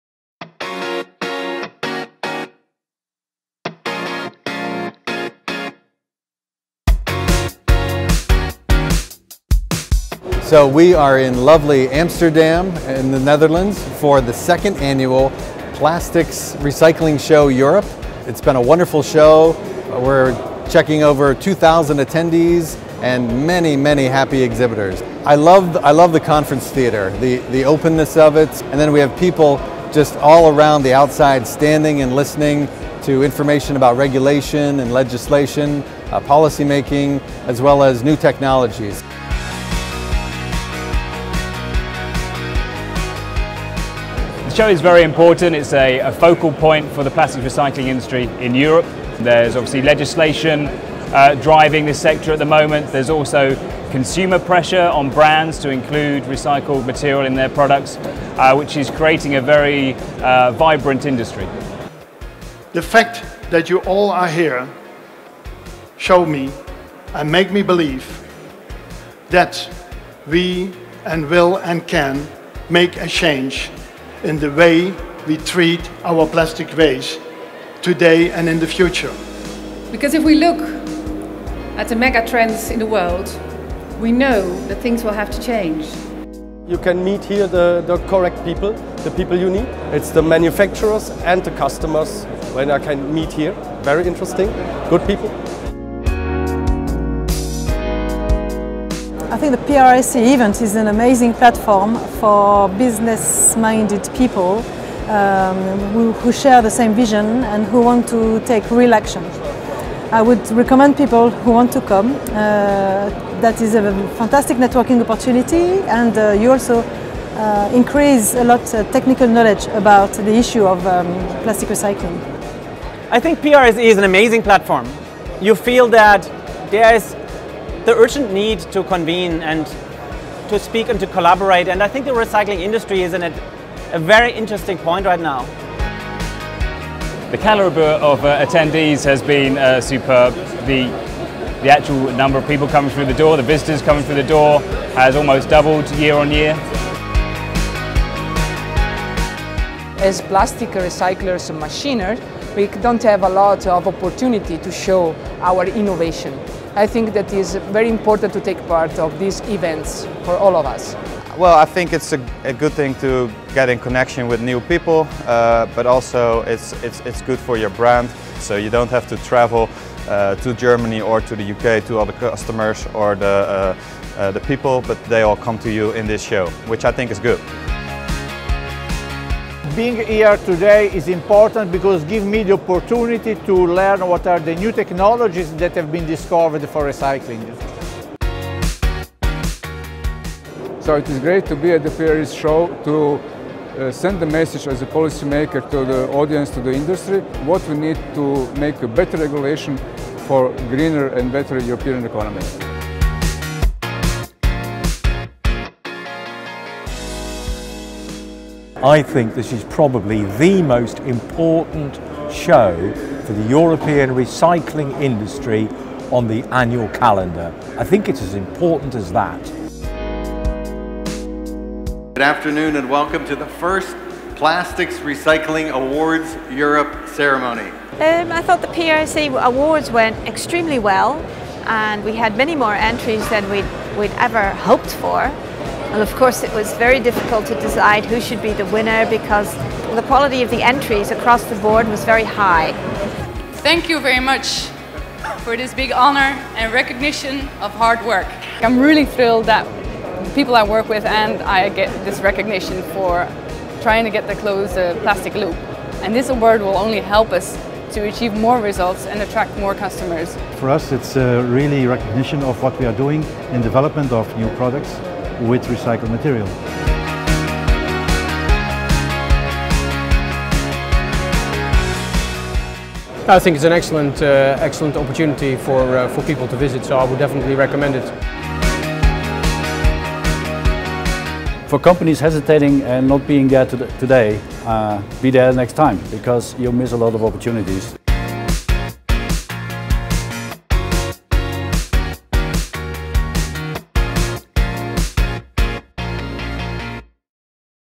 PRSE 2018 Highlights Film
The free to attend conference and exhibition has firmly established itself as the focal point of plastics recycling in Europe. The biggest names in recycled materials, recycling machinery and services will be on display, showing the latest innovation from this exciting industry.

The informative and inspiring free conference with key industry figures will address the latest opportunities and challenges that face the plastic recycling industry in Europe.

If you are a brand owner, retailer, converter, packaging manufacturer or consumer of recycled material then this event is for you!